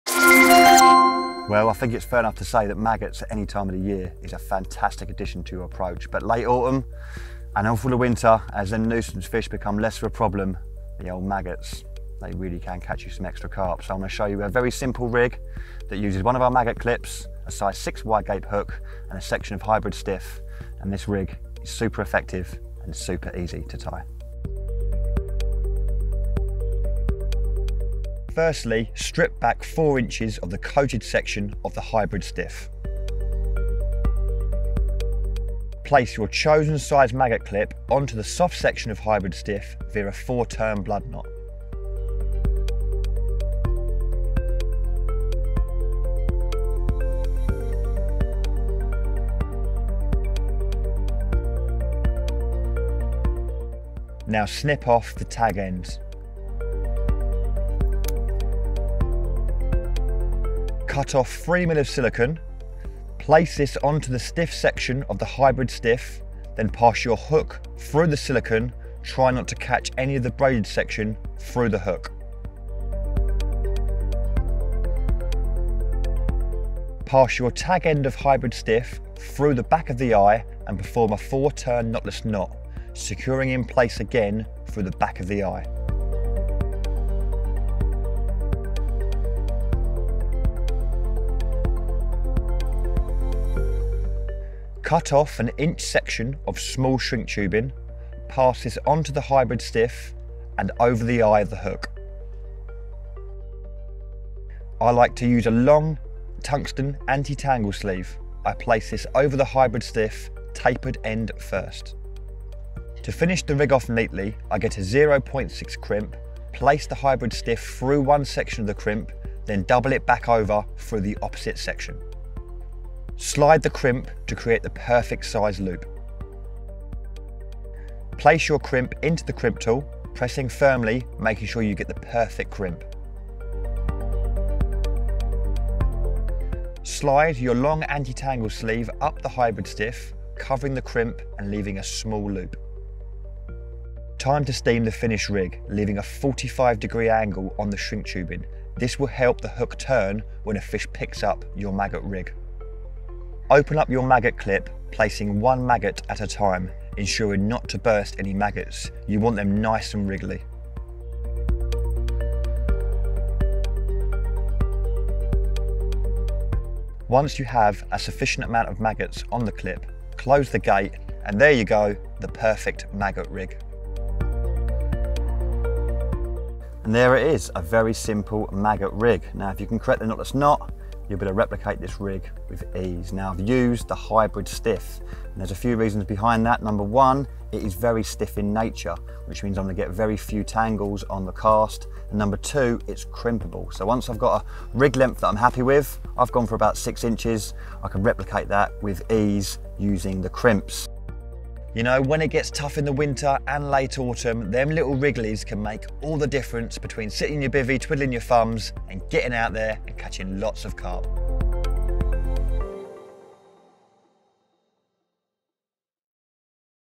How To Tie Maggot Clip Rig | Korda Carp Fishing
There’s no doubting Maggots are a great bait for catching Carp, year-round.

Korda Koach, Rob Burgess, shows you how to tie a simple, yet extremely effective rig that is perfect for presenting a big bunch of Maggots, with the use of one of our handy Maggot Clips.